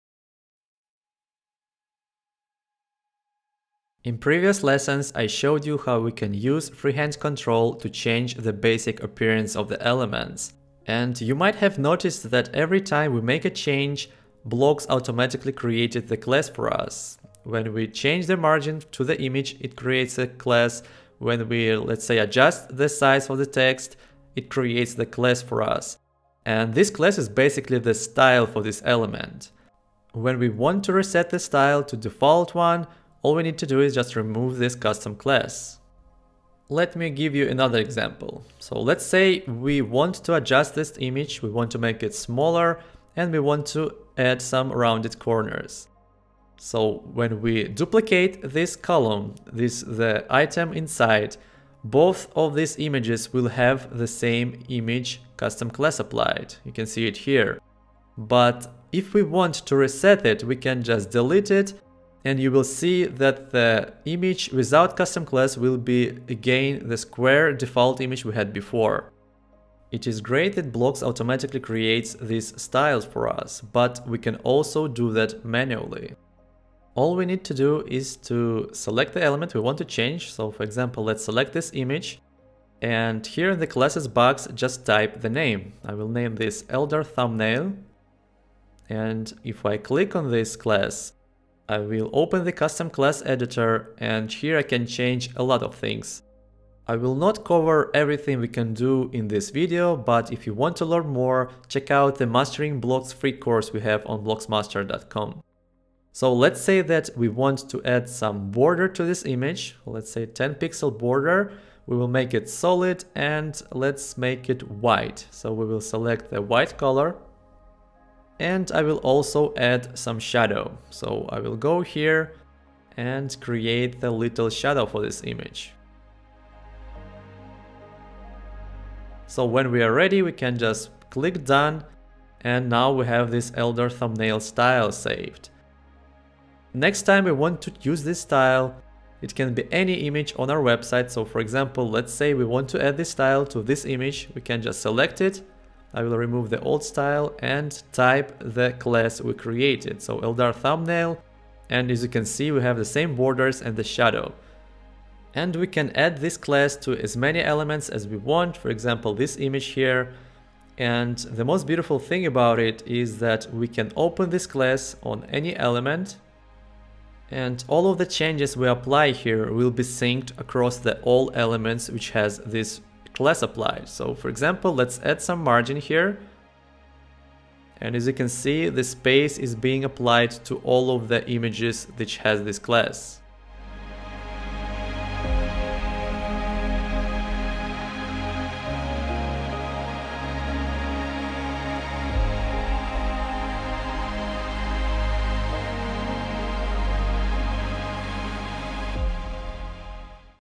Understanding Custom Classes in Blocs 3
This video is a part of new 'Getting Started with Blocs 3' video course, which is available for free on Blocs Master website.

 I have created this course to help new Blocs users understand the basics about Blocs 3.